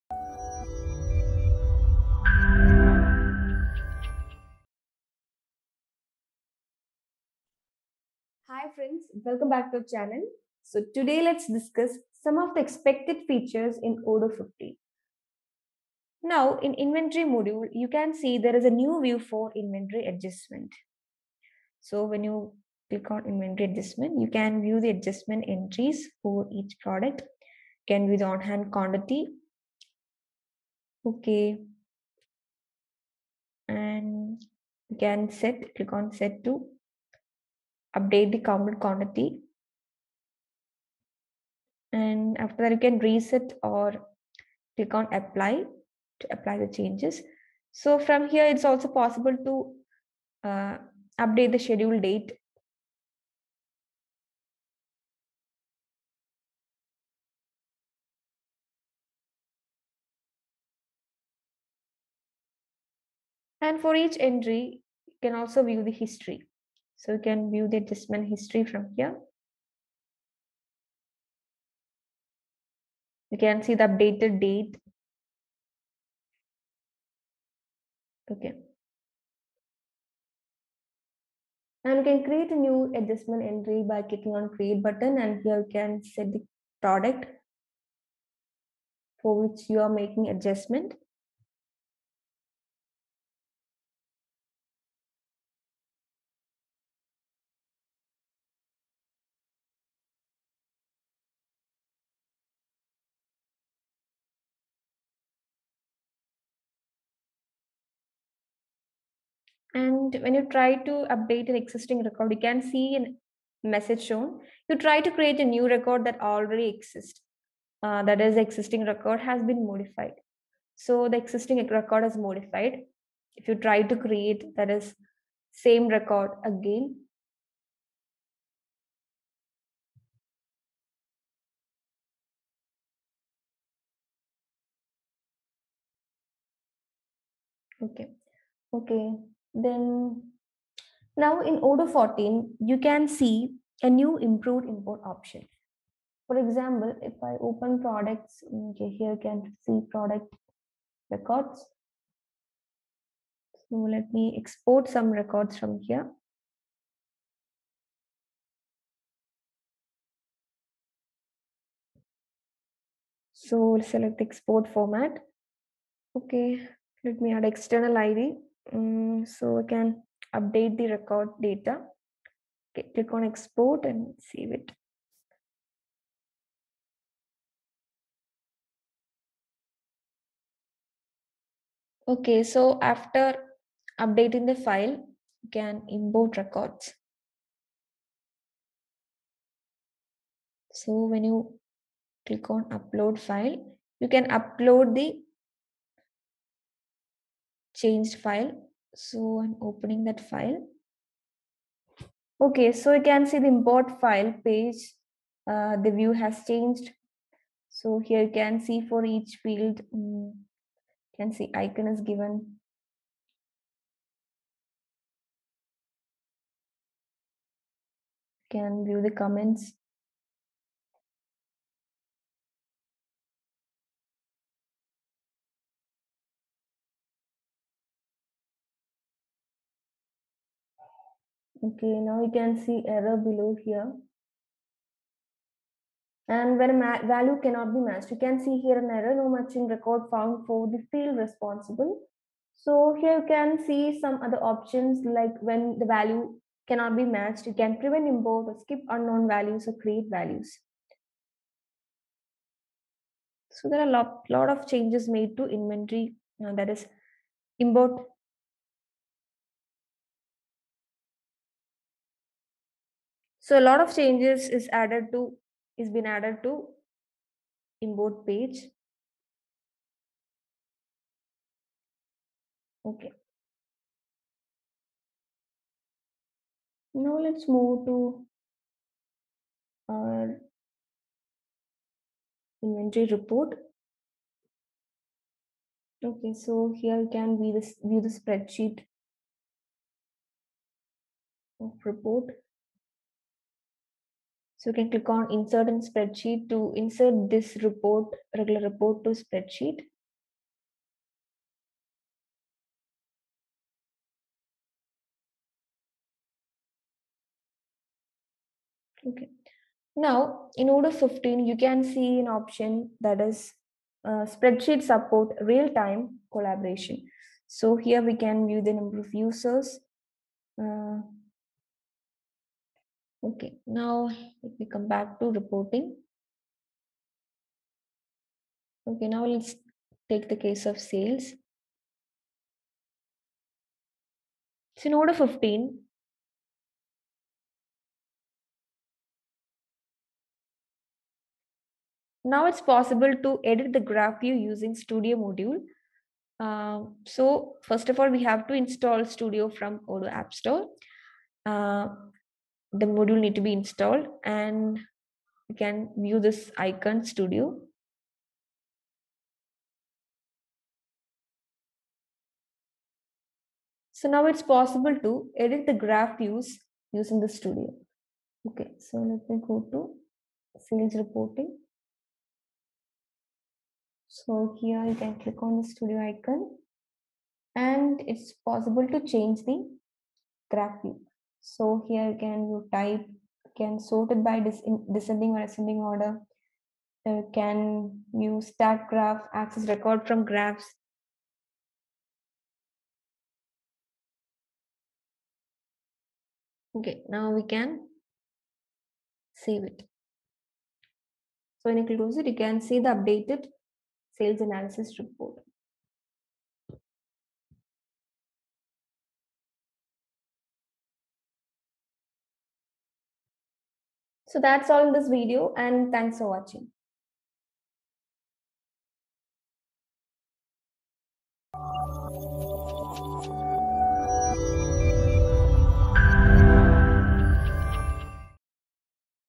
Odoo 15 Expected Features: What's New? | Odoo Experience 2021
In this video, we are going to discuss some of the expected features in Odoo 15 which is expected to be released in October 2021. Start your journey on Odoo 15,

0.00 Intro - Odoo15Features What's New?
2:18 - New Improved import option
4:34 - Real-time Collaboration in Spreadsheet
5:27 - Ability to Edit Graph Views Using Studio